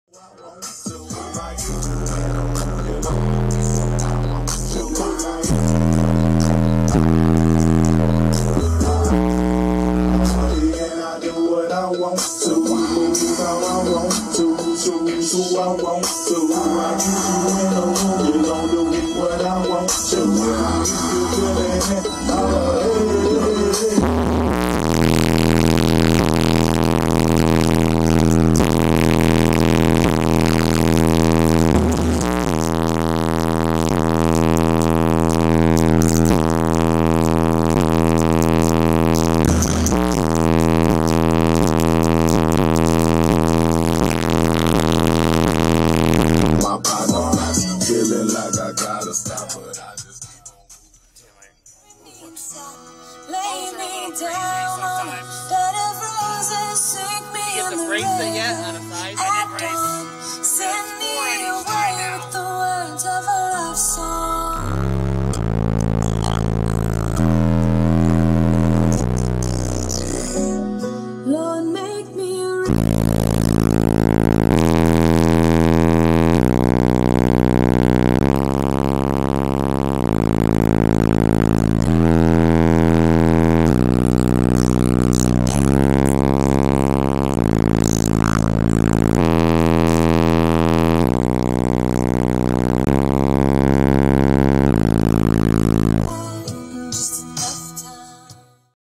Mike's BAD ASS Trailblazer
Got the 2nd 8k in and it's way louder and windier now!
(8) DC xL 15s
(2) Crossfire 8ks
XS Power d3100s
Singer Alts
TDHFTMFW